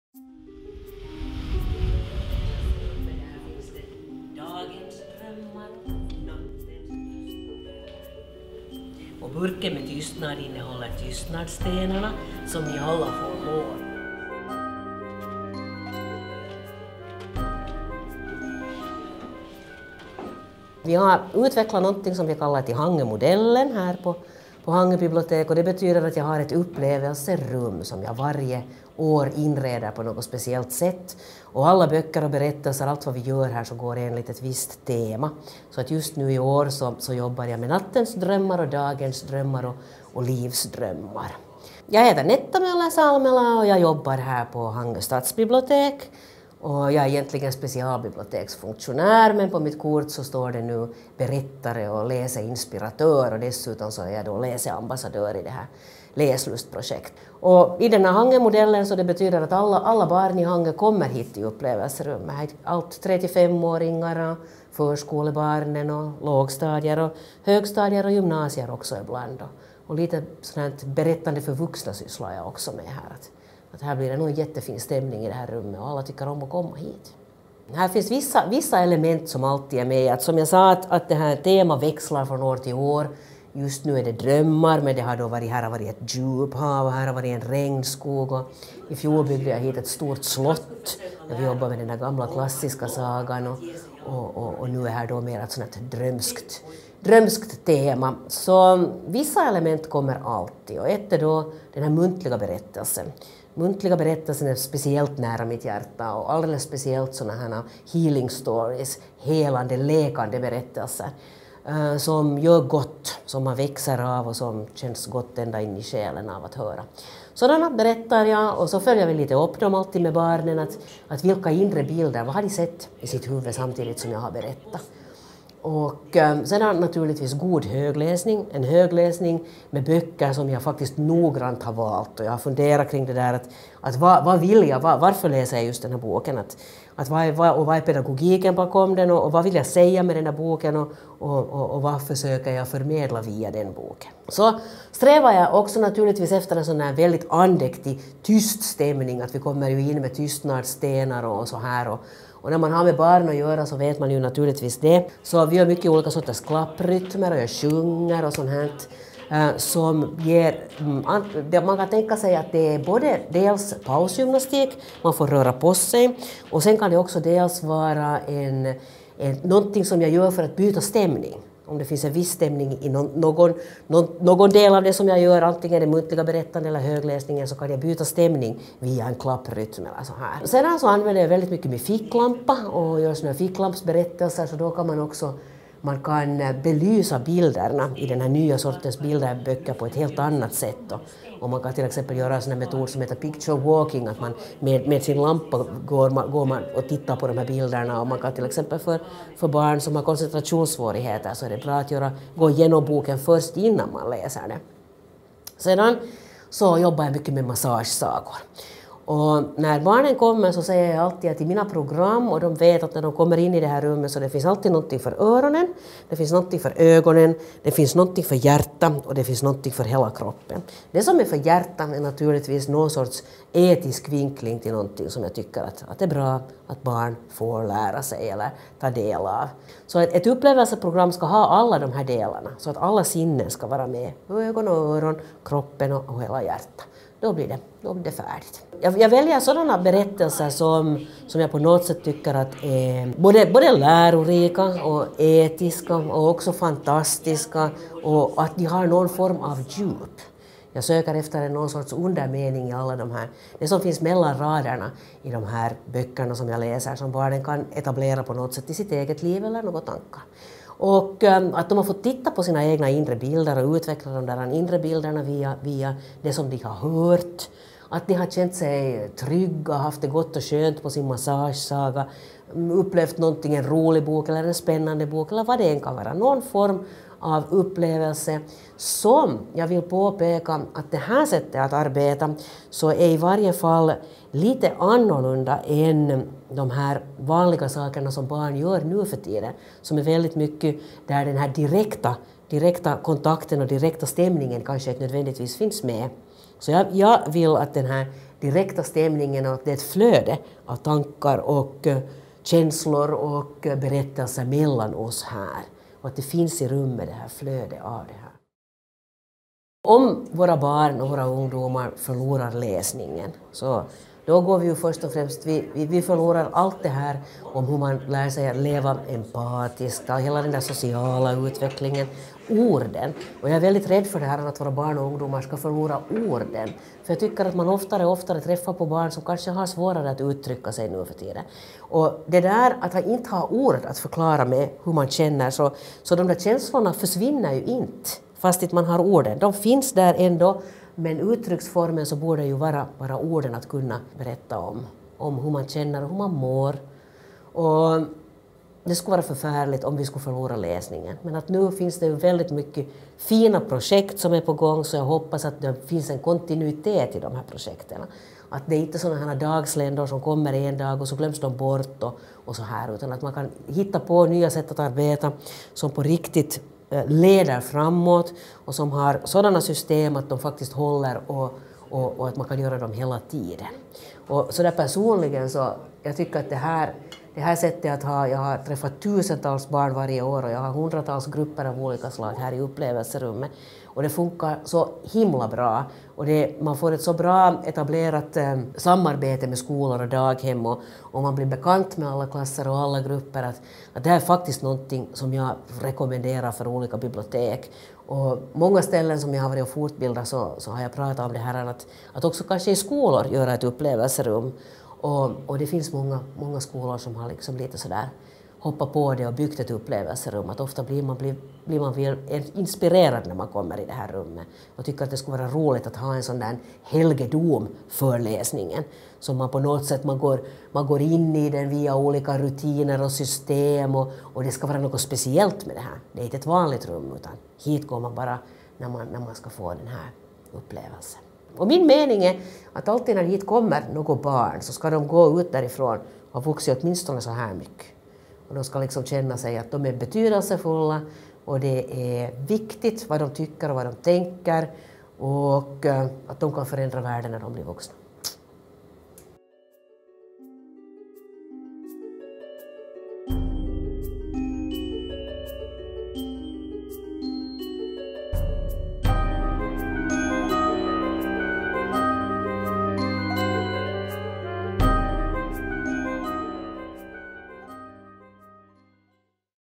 Läsinspiratören Agneta Möller-Salmela berättar om sina metoder
Agneta (Netta) Möller-Salmela är specialbiblioteksfunktionär, berättare, läsinspiratör och dessutom läsambassadör i projektet Läslust. Hon har utvecklat nya metoder för läsinspiration och har länge arbetet med att skapa det optimala samarbetet mellan biblioteket, skolorna och hemmen. Varje år träffar hon tusentals barn och unga i olika åldrar.

På videon får du följa med in i Agneta Möller-Salmelas fantasifullt inredda upplevelserum på biblioteket i Hangö och höra henne berätta om sina metoder. Upplevelserummet inreds varje år enligt ett nytt tema. Just nu är temat nattens drömmar, dagdrömmar och livsdrömmar.

Agneta Möller-Salmela både berättar muntliga berättelser och läser högt ur böcker. Hennes program innehåller alltid något för så väl öronen som för ögonen, hjärtat och hela kroppen. På videon får du se och höra hur hon i sina program använder sig av bl.a. ficklampa, tystnadsstenar, klapprytmer och massage.

Intervjuare: Riitta Taarasti
Film: Panu Somerma och Mikko Helander
Klipp: Panu Somerma

Produktion: Bibliotekskanalen – Biblioteken.fi, 2014